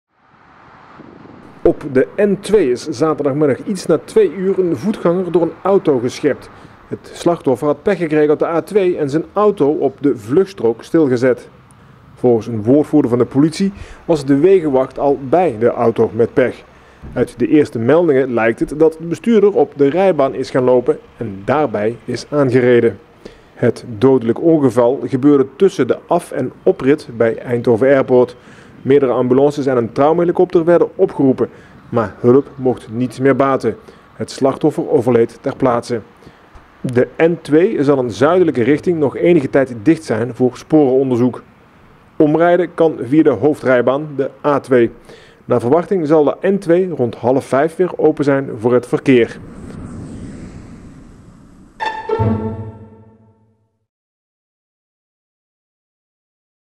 Voetganger overleden na aanrijding met auto bij oversteken N2
Op de N2 is zaterdagmiddag iets na twee uur een voetganger door een auto geschept. Het slachtoffer had pech gekregen op de A2 en had zijn auto op de vluchtstrook stil gezet. De man is ter plaatse overleden. Dat meldt de politie.